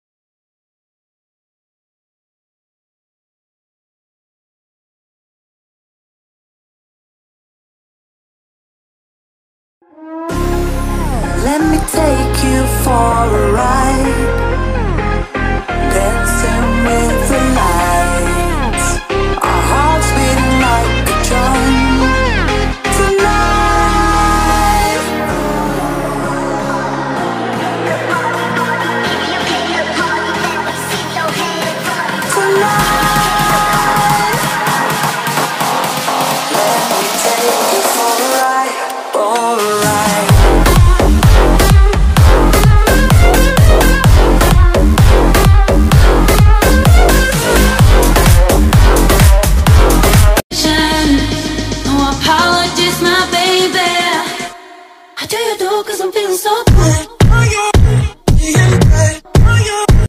Marinette goes back in time when her parents were dating- | Miraculous ladybug [MLB] | Gacha Club
Video song -NCS: Music Without Limitations